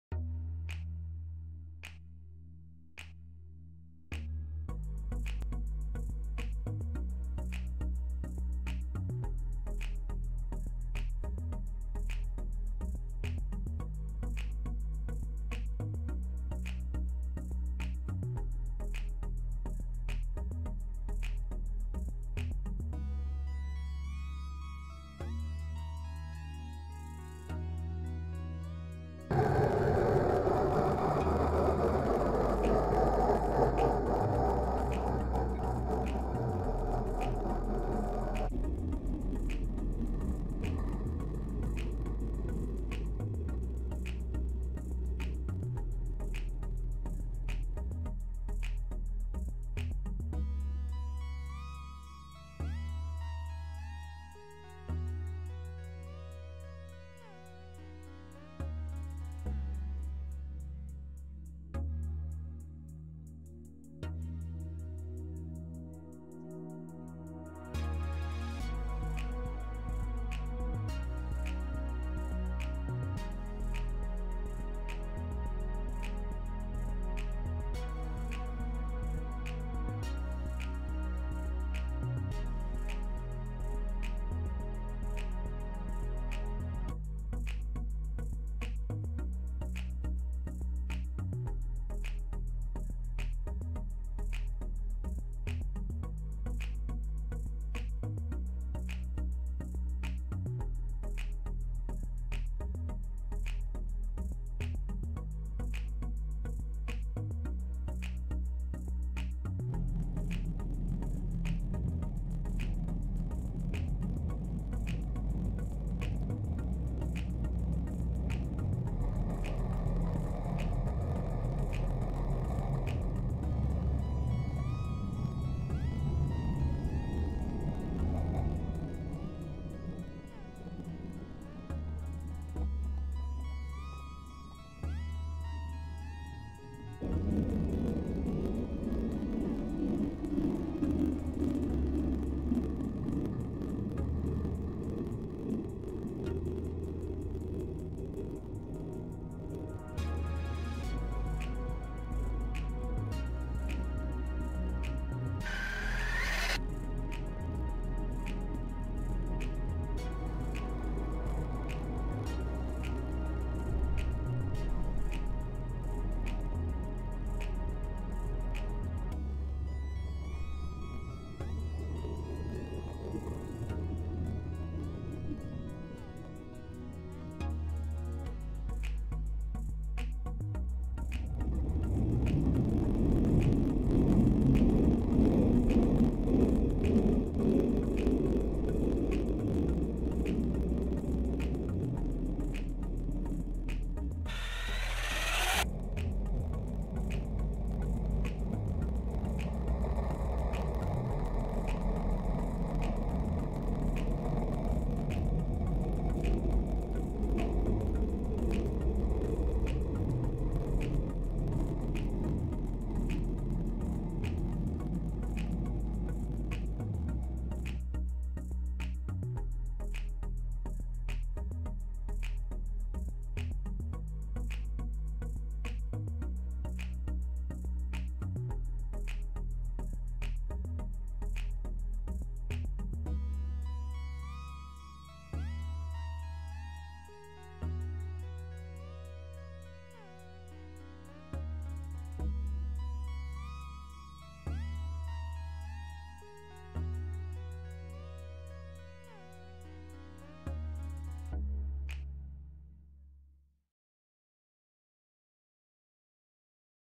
Traxxas Stampede 4s lipo cruising
The lake was frozen, so i thought about going for a ride :)